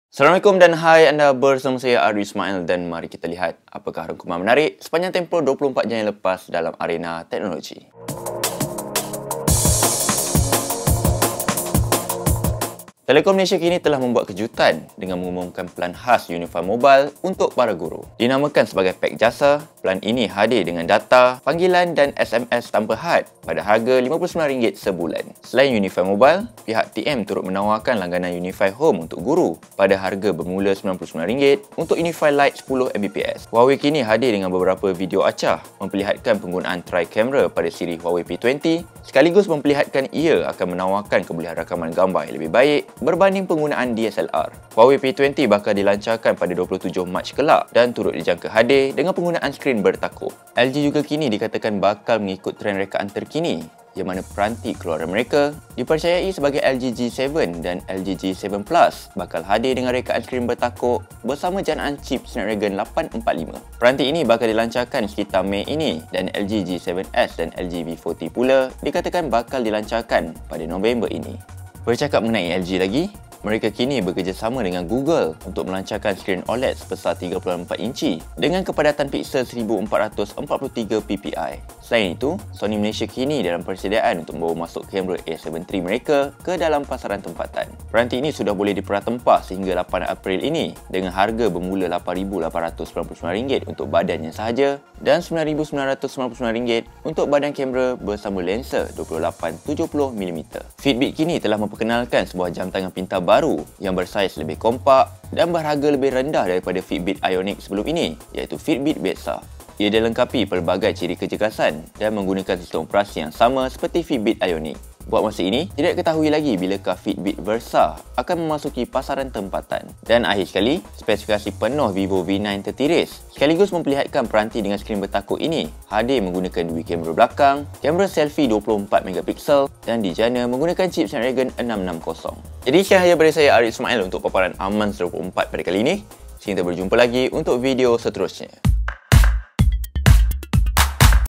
Amanz24 - Pelan unifi Untuk Guru, LG G7 Bakal Hadir, Fitbit Versa Dilancarkan
Telekom Malaysia kini membuat kejutan dengan mengumumkan pelan khas Unifi Mobile untuk para guru. Dinamakan sebagai Pek Jasa, pelan ini hadir dengan data, panggilan dan SMS tanpa had pada harga RM59 sebulan. Selain Unifi Mobile, pihak TM turut menawarkan langganan Unifi Home untuk guru pada harga langganan bermula RM99 untuk Unifi Lite 10Mbps.

Huawei kini hadir dengan beberapa video acah, memperlihatkan penggunaan tri-kamera pada siri Huawei P20, sekaligus memperlihatkan ia akan menawarkan kebolehan rakaman gambar yang lebih baik berbanding penggunaan DSLR. Huawei P20 bakal dilancarkan pada 27 Februari kelak, dan turut dijangka hadir dengan penggunaan skrin takuk.

LG juga kini dikatakan bakal mengikut trend rekaan terkini dimana peranti keluaran mereka, dipercayai sebagai LG G7 dan LG G7 Plus bakal hadir dengan rekaan skrin takuk bersama janaan cip Snapdragon 845. Peranti ini bakal dilancarkan sekitar Mei ini dan LG G7S dan LG 40 pula dikatakan bakal dilancarkan pada November ini.

Bercakap mengenai LG lagi, mereka kini bekerjasama dengan Google untuk melancarkan skrin OLED sebesar 3.4 inci dengan kepadatan pixel 1443 PPI. Peranti ini dipercayai bakal dilancarkan pada Mei ini di acara DisplayWeek kelak.

Selain itu, Sony Malaysia kini dalam persediaan untuk membawa masuk kamera A7 III mereka dalam pasaran tempatan. Peranti ini sudah boleh dipra-tempah sehingga 8 April ini dengan harga bermula RM8899 untuk badannya sahaja, dan RM9999 untuk badan kamera bersama lensa 28-70mm.

Fitbit terkini telah memperkenalkan sebuah jam tangan pintar baru yang bersaiz lebih kompak dan berharga lebih rendah daripada Fitbit Ionic sebelum ini, iaitu Fitbit Versa. Ia dilengkapi pelbagai ciri kecergasan, dan menggunakan sistem operasi sama seperti pada Fitbit Ionic. Buat masa ini, tidak diketahui lagi bilakah Fitbit Versa akan memasuki pasaran tempatan.

Dan akhir sekali, spesifikasi penuh Vivo V9 tertiris, sekaligus memperlihatkan peranti dengan skrin takuk berkenaan hadir menggunakan dwi-kamera belakang, kamera swafoto 24MP, dan dijana menggunakan cip Snapdragon 660.